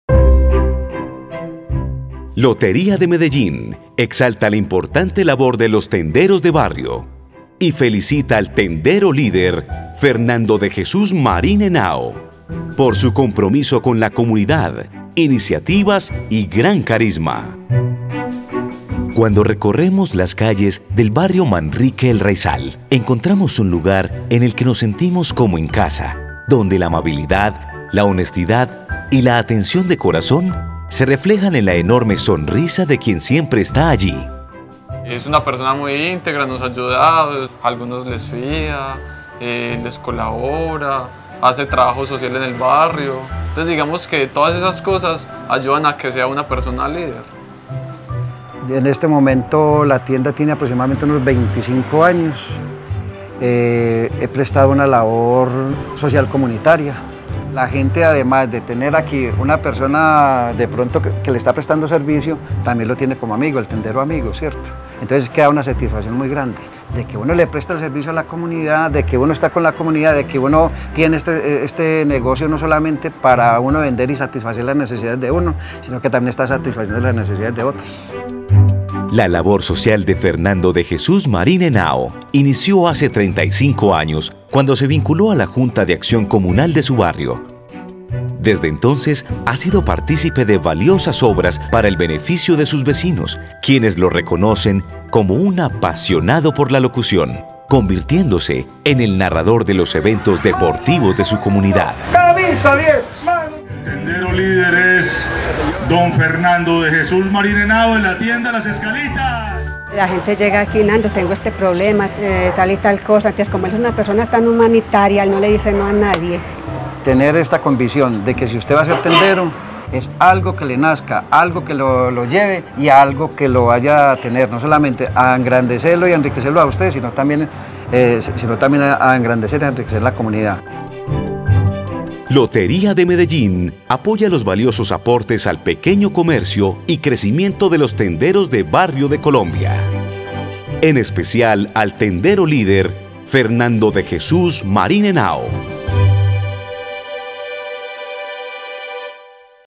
Tendero Líder 2015, Fernando de Jesús Marín Henao
Fernando de Jesús Marín Henao de la tienda "FM Las Escalitas" del barrio El Raizal, Manrique, es el Tendero Líder 2015. Reconocido en La Noche de los Mejores 2015.